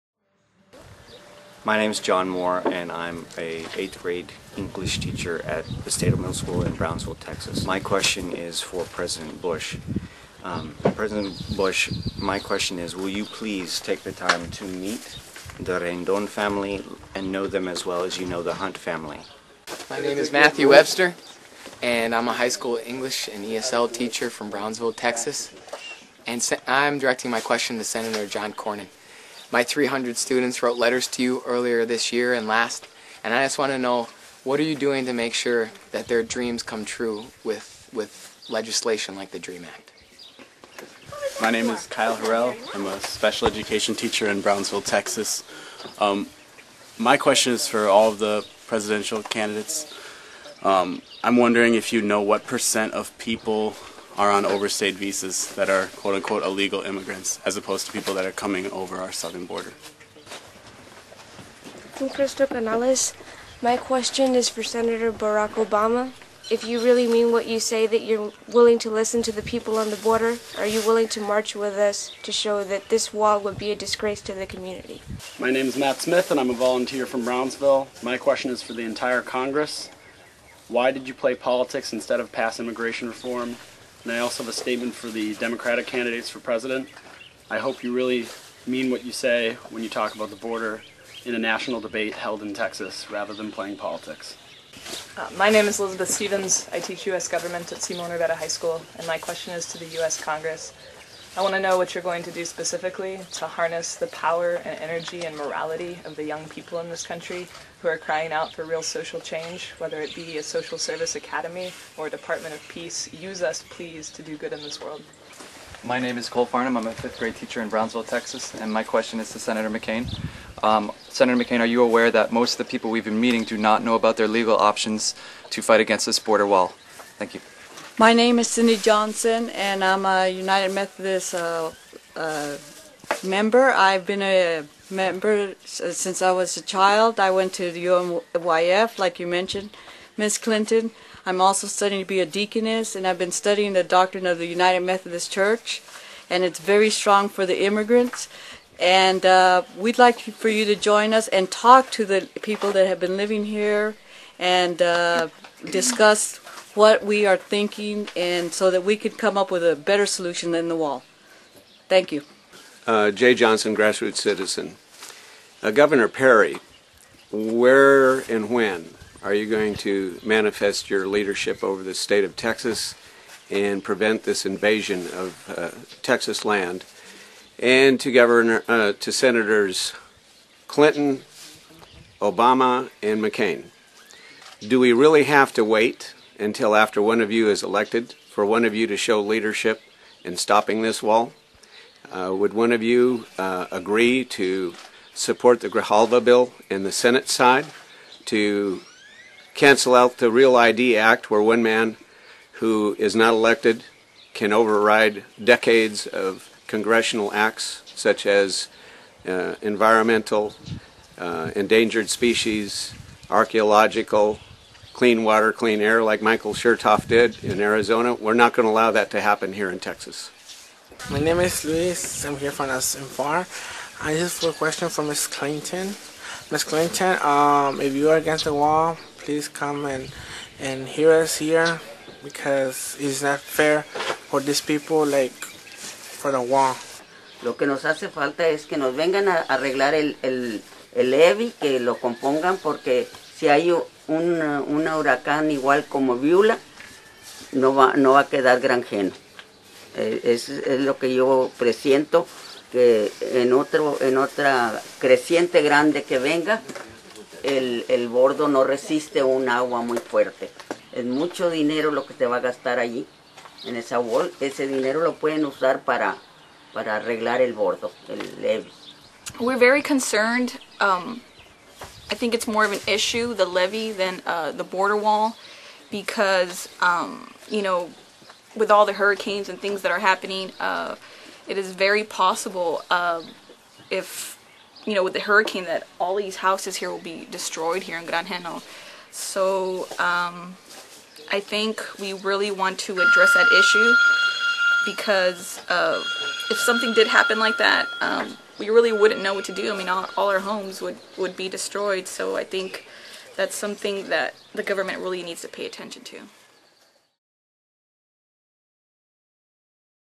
My Question Is
In Granjeno, Texas, where the Department of Homeland Security seeks to reconstruct a levee to include a 16foot concrete wall, citizens against border barriers pose their questions to current political leaders and Presidential candidates.

Citizens protesting the border fence pose questions